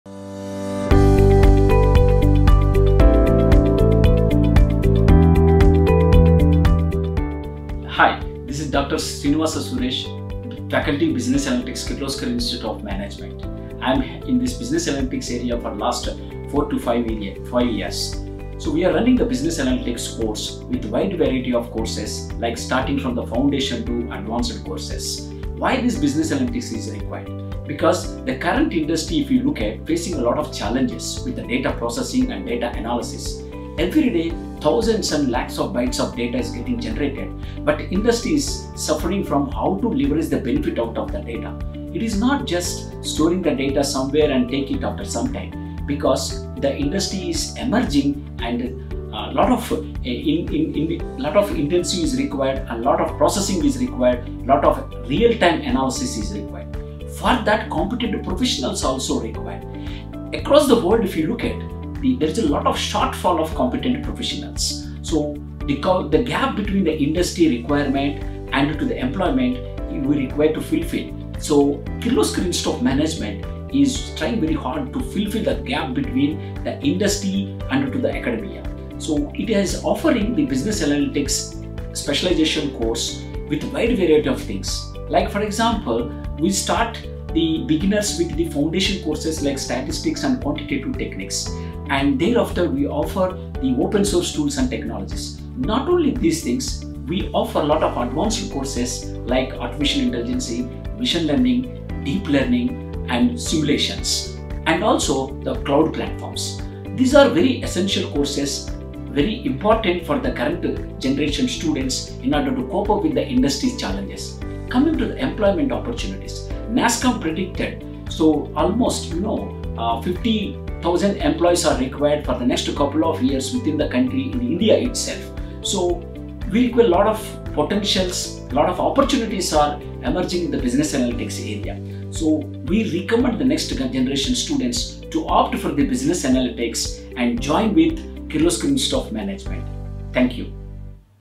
Meet Dr. Srinivasa Suresh, Associate Professor at the Kirloskar Institute of Management.
In the latest episode of the KIM faculty podcast, Dr. Srinivasa Suresh shares his views about the growing demand for data analytics and how KIM is providing an incredible platform for students to establish themselves as Industry-ready professionals.

"NASSCOM has evaluated huge employment opportunities for Data analytics professionals in the next 50 years within India itself. The talented and skilled data analytics candidates can help industries leverage the power of data to make informed decisions, optimize operations, enhance customer experience, foster innovation, and build a data-driven culture. At Kirloskar Institute of Management, we have established Data Analytics programs keeping in mind the evolving needs of the Industry" - Dr. Suresh, Associate Professor, KIM.

Dr. Suresh has been serving the education domain for the last 20 years. When it comes to teaching, his areas of interest include Business Analytics, Social Media Analytics, Database Management System, Machine Learning and Data Mining!